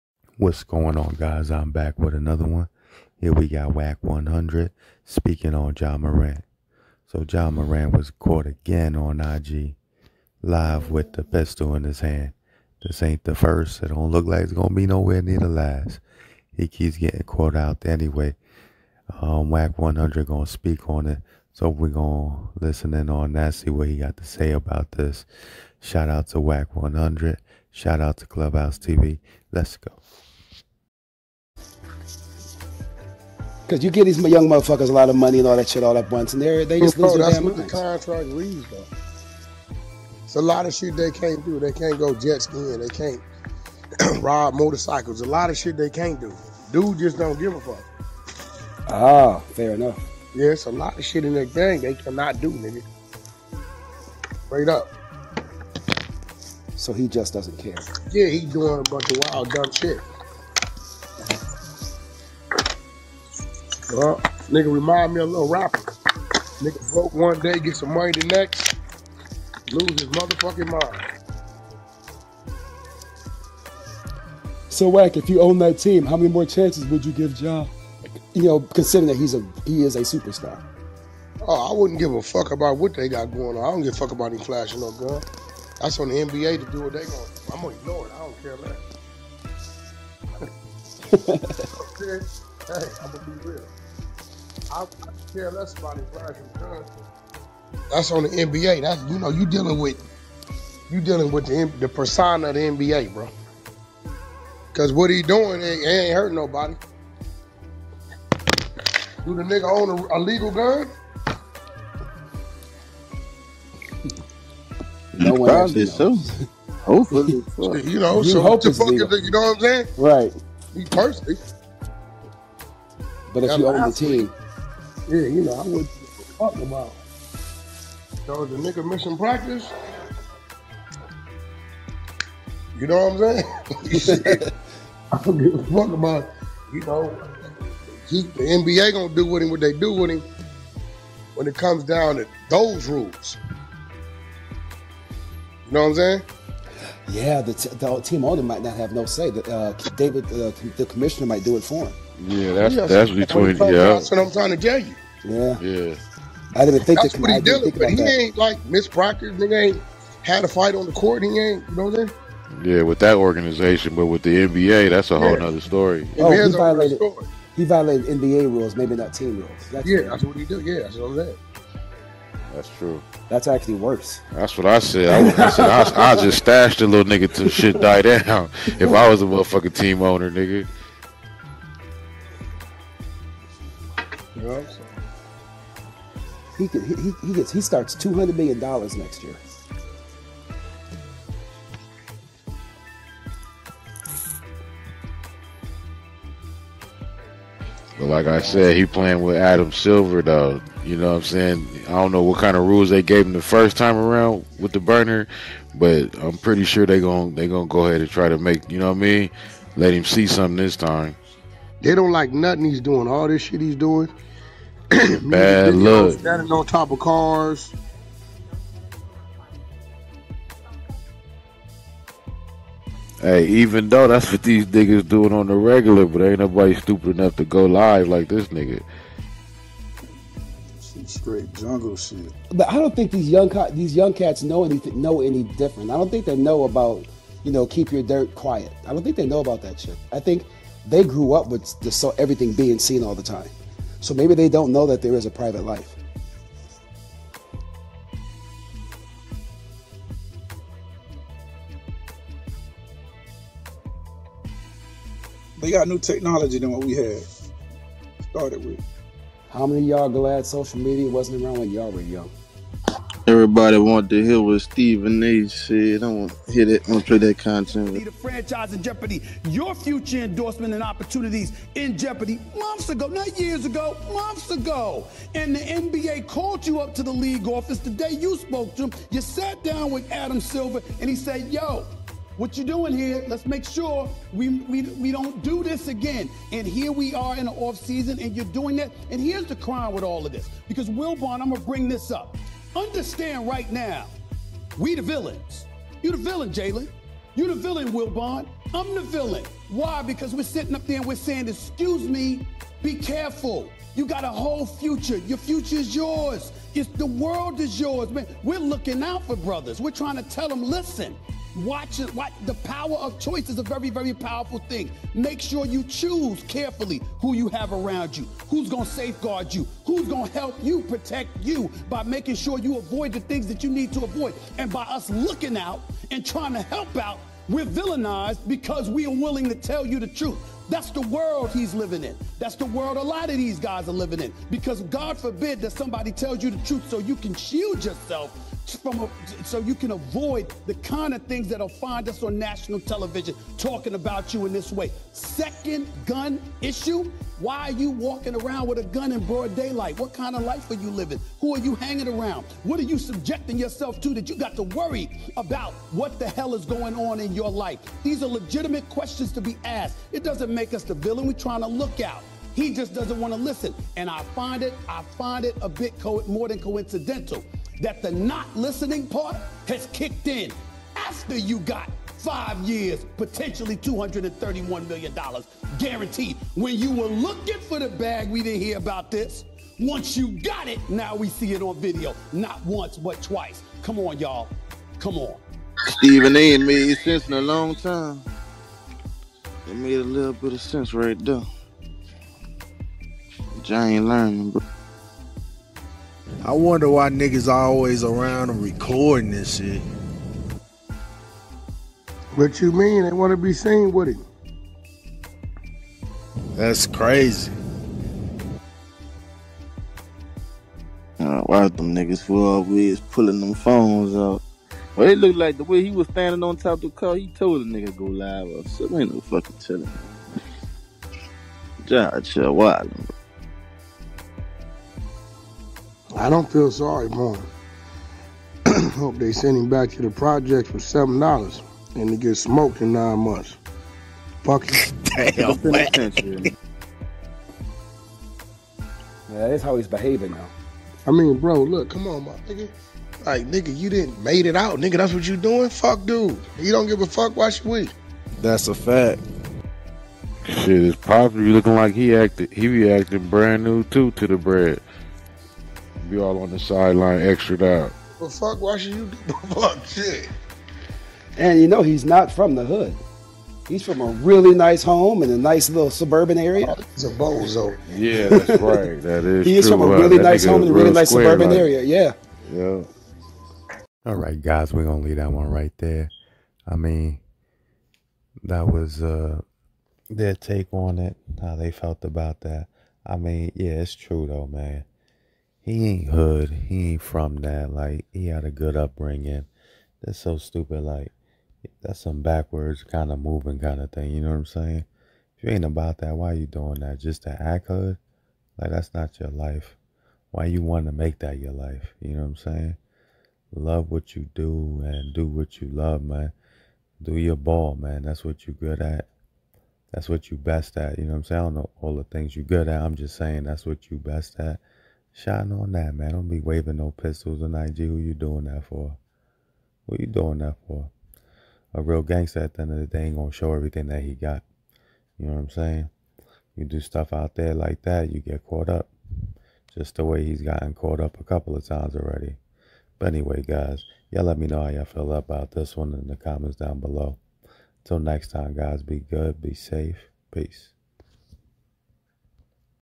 Wack 💯 Speaks On Ja Morant going on ig live showing off gun again
wack 💯 Speaks On ja morant for showing off gun on ig Live again entertainment wack100 clubhouse jamorant .
1)This video has no negative impact.
3)It is transformative in nature.